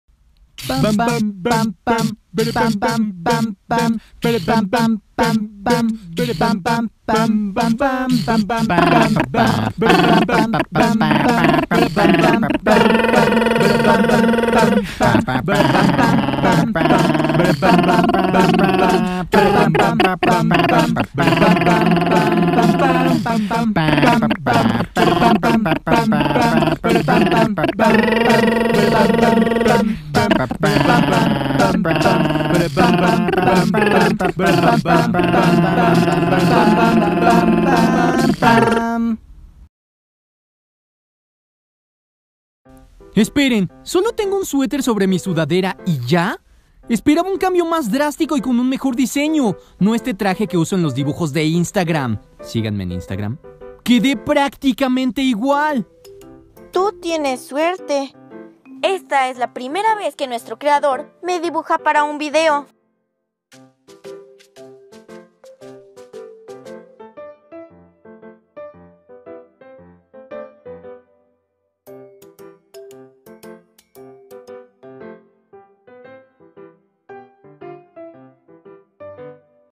Mi Nuevo Look
Creo que no me había tomado el tiempo de explicar porque el cambio de apariencia de Ryusakipool... y la verdad es que no hay mucho que explicar, sólo me gustó usar un suéter negro que tenía por ahí sobre mi sudadera y ya (?)